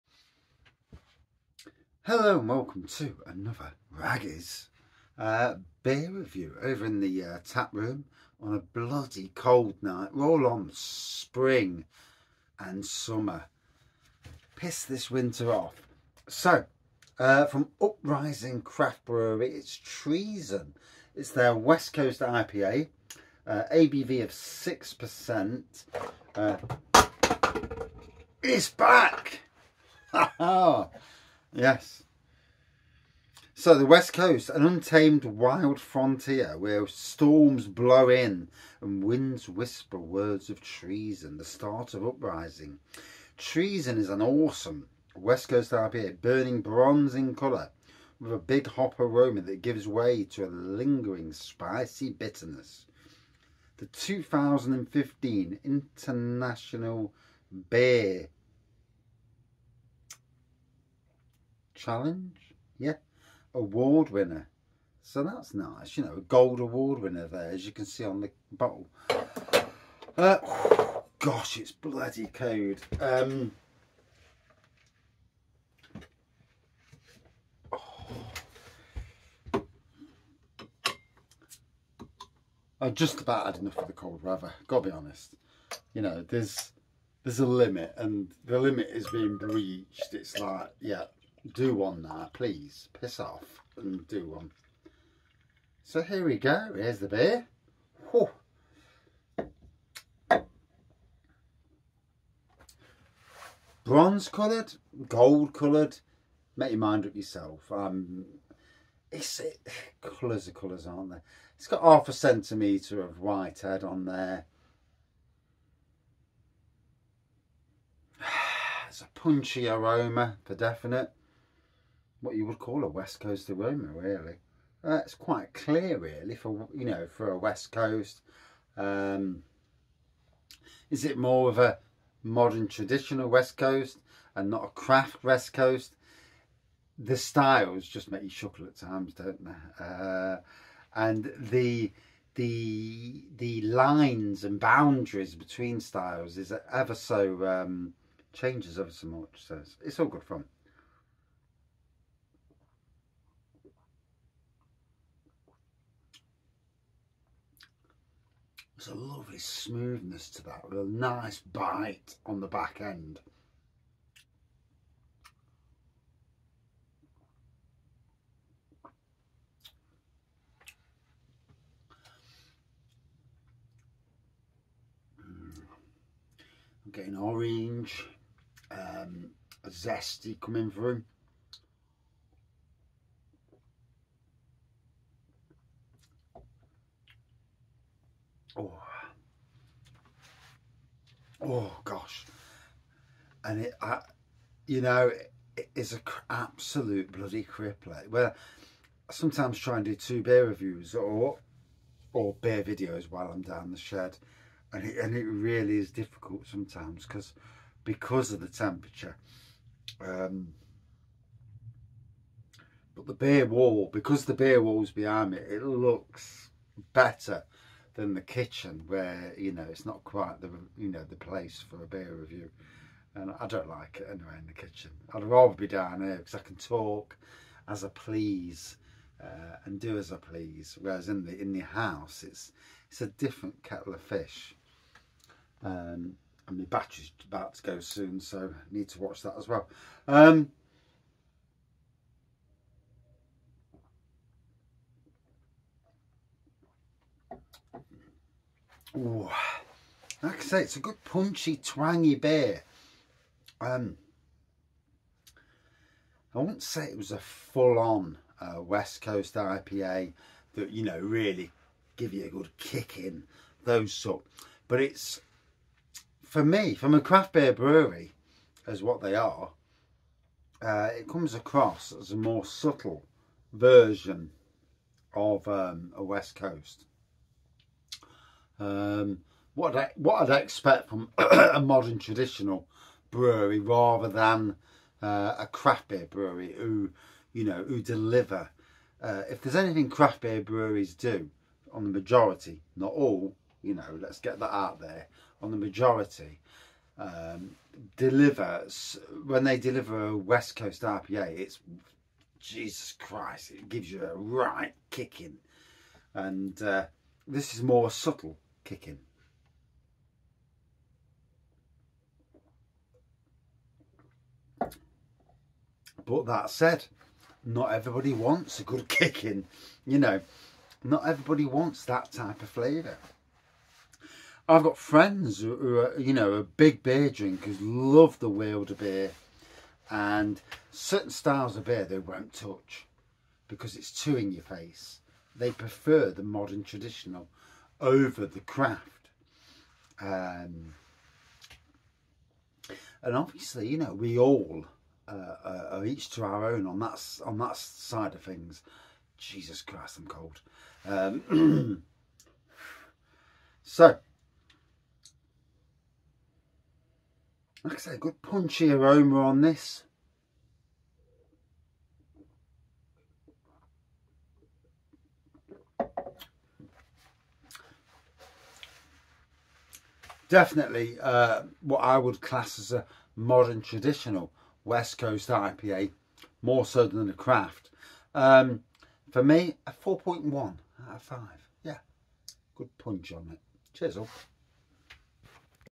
Uprising Brewery Treason West Coast IPA - Craft Beer Review
Over to Uprising Craft Brewery for a West Coast IPA from them, What we have is a Punchy number and as usual you'll get a wraggy review with tangents, heres my review